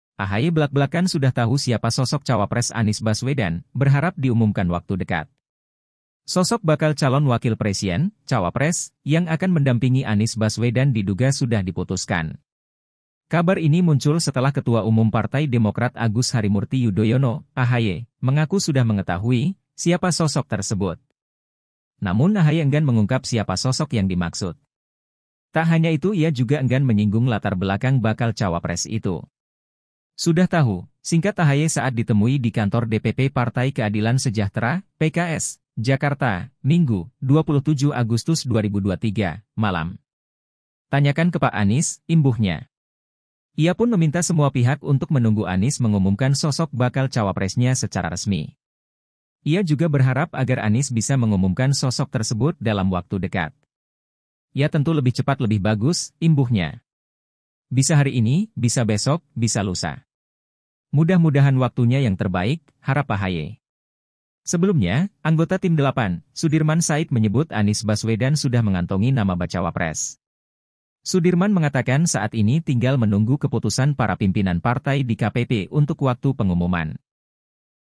AHY Blak-blakan Sudah Tahu Siapa Sosok Cawapres Anies Baswedan, Berharap Diumumkan Waktu Dekat
TRIBUNSOLO.COM - Sosok bakal calon wakil presien (cawapres) yang akan mendampingi Anies Baswedan diduga sudah diputuskan.

Kabar ini muncul setelah Ketua Umum Partai Demokrat Agus Harimurti Yudhoyono (AHY) mengaku sudah mengetahui, siapa sosok tersebut.

Namun AHY enggan mengungkap siapa sosok yang dimaksud.

Tak hanya itu ia juga enggan menyinggung latar belakang bakal cawapres itu.

"Sudah tahu," singkat AHY saat ditemui di Kantor DPP Partai Keadilan Sejahtera (PKS), Jakarta, Minggu (27/8/2023) malam.

"Tanyakan ke Pak Anies," imbuhnya.
Ia pun meminta semua pihak untuk menunggu Anies mengumumkan sosok bakal cawapresnya secara resmi.

Ia juga berharap agar Anies bisa mengumumkan sosok tersebut dalam waktu dekat.

"Ya tentu lebih cepat lebih bagus," imbuhnya.

"Bisa hari ini, bisa besok, bisa lusa. Mudah-mudahan waktunya yang terbaik," harap AHY.

Sebelumnya, Anggota Tim 8, Sudirman Said menyebut Anies Baswedan sudah mengantongi nama bacawapres.

Sudirman mengatakan saat ini tinggal menunggu keputusan para pimpinan partai di KPP untuk waktu pengumuman.

VP: TEGAR